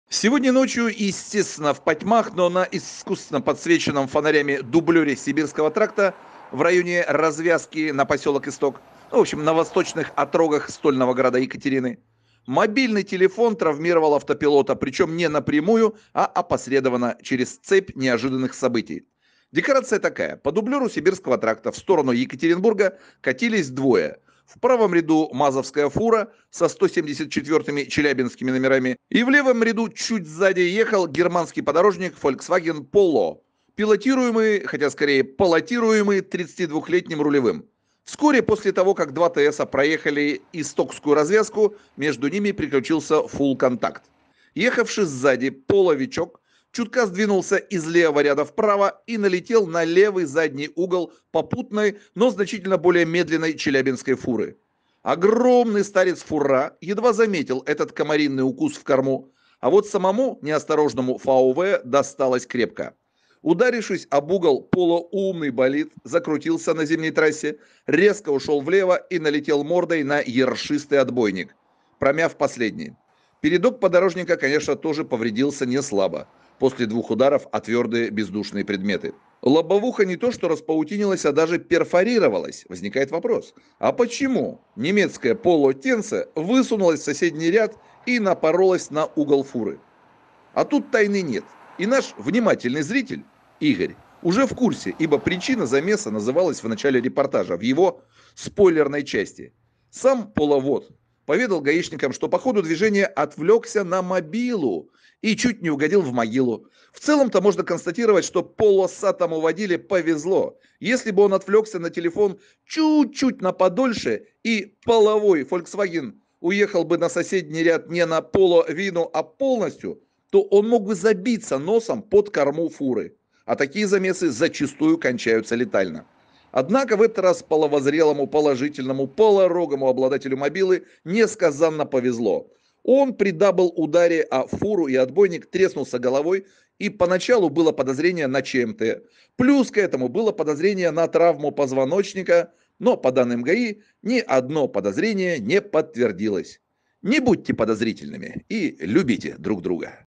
"Поло" раскрошил морду о фуру и отбойник
"Фольксваген" мог забиться под корму грузовика, но ему повезло